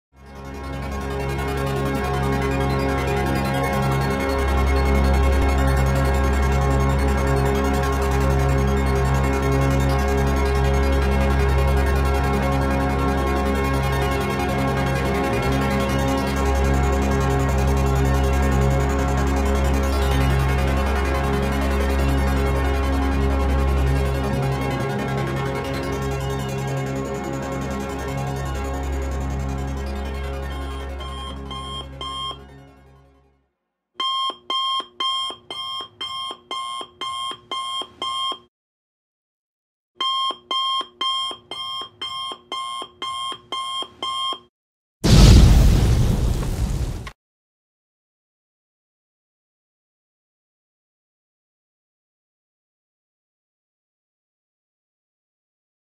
pouso forçado - os quero quero queria derrubar meu drone
pensei que eu tranquilo mas derrepente veio uma chuva de quero queropra cima do drone, a imagem deles vindo pra cima do drone poderia ser melhor mas me apavorei era muitos